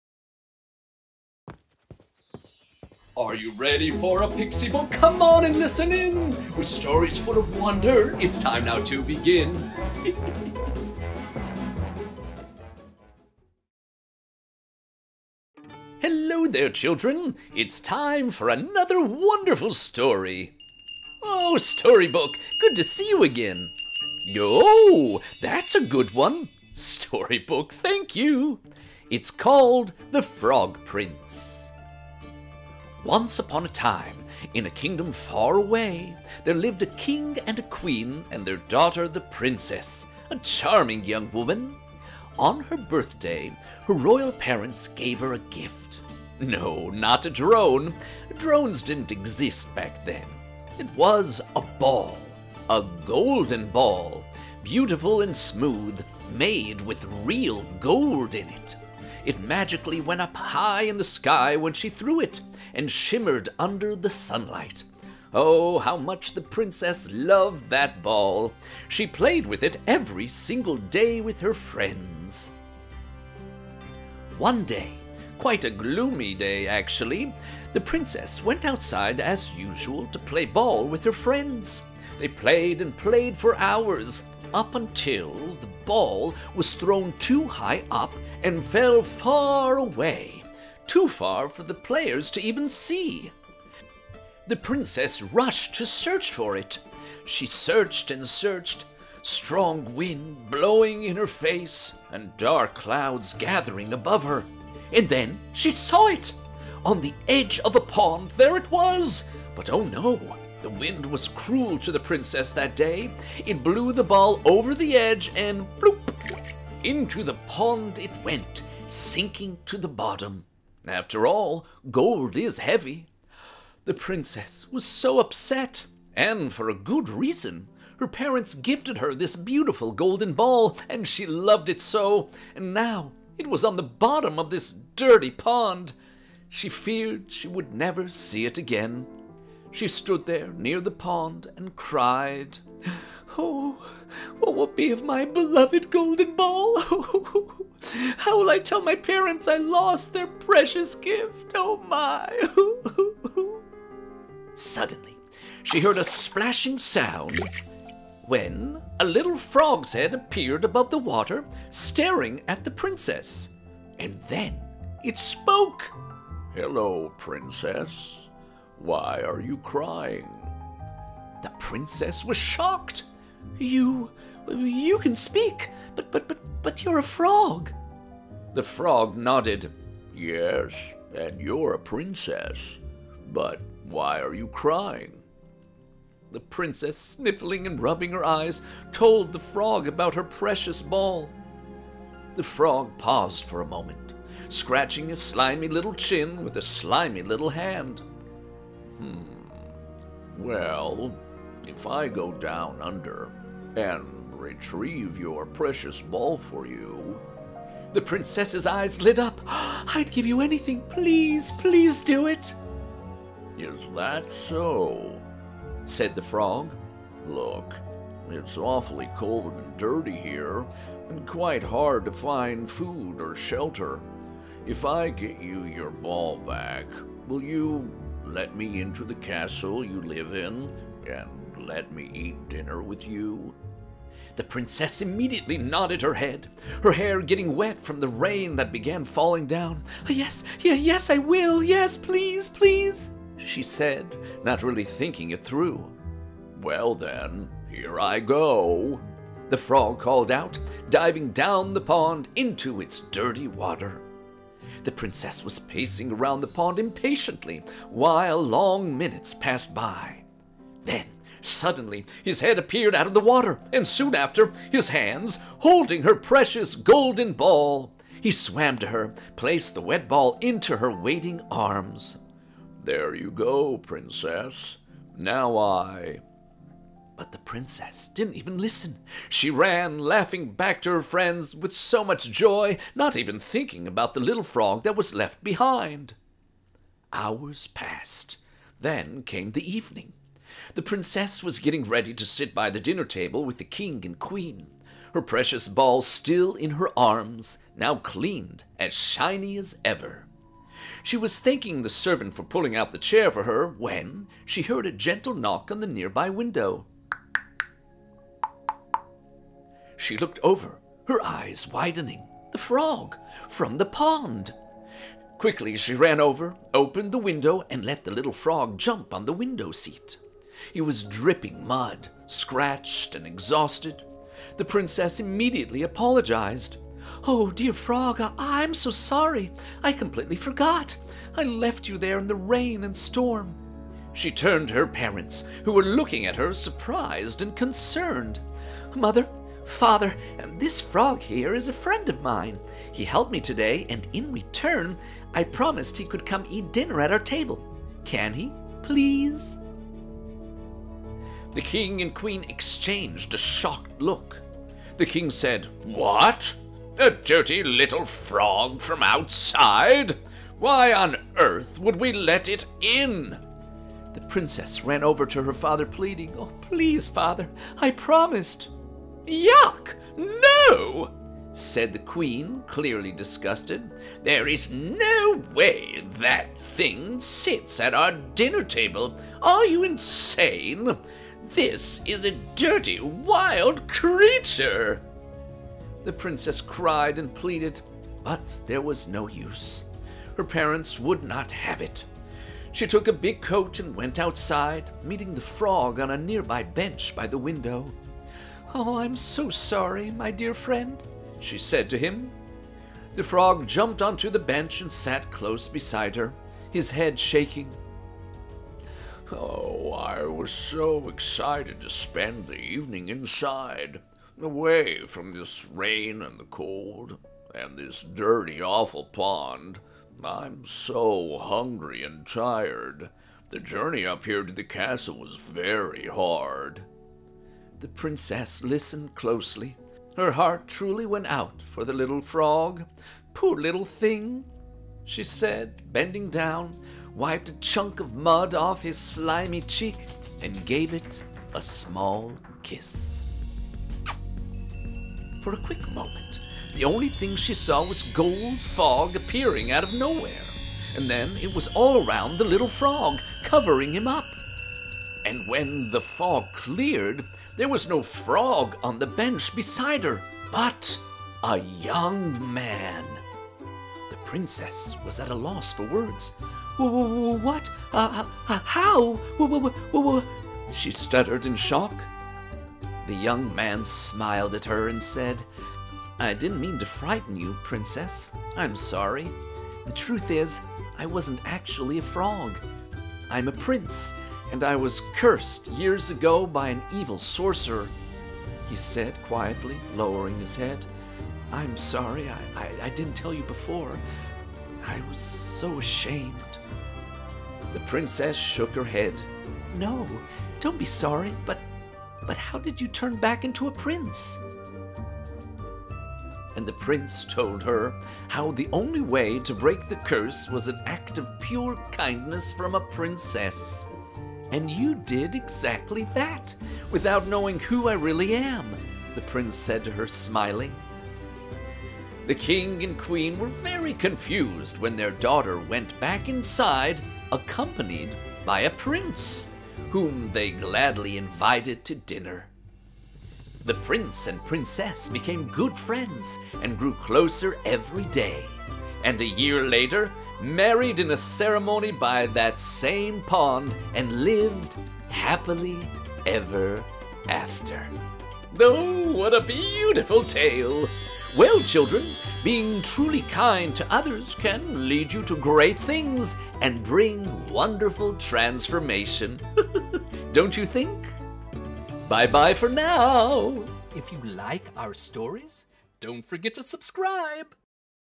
The Frog Prince | Bedtime Story | PixieBooks
PixieBooks tells the classic fairy tale of The Frog Prince. The story revolves around a princess who loses her beloved golden ball in a pond, and a friendly frog offers to retrieve it for her. The frog asks the princess to let him eat dinner with her in the castle in return for his help. The princess agrees, but once she gets her ball back, she forgets about the frog and runs away without keeping her promise. Later, the frog appears at the castle, seeking to be let in and dine with the princess, but the king and queen refuse, considering him a dirty, wild animal. The princess then goes to meet the frog outside, apologizes for breaking her promise, and sits with him on a bench to keep him company. Apparently, the frog was actually a cursed prince and the princes’ act of pure kindness broke the curse. The story teaches us the importance of keeping our word and treating everyone, even those who may seem insignificant or different, with kindness and respect.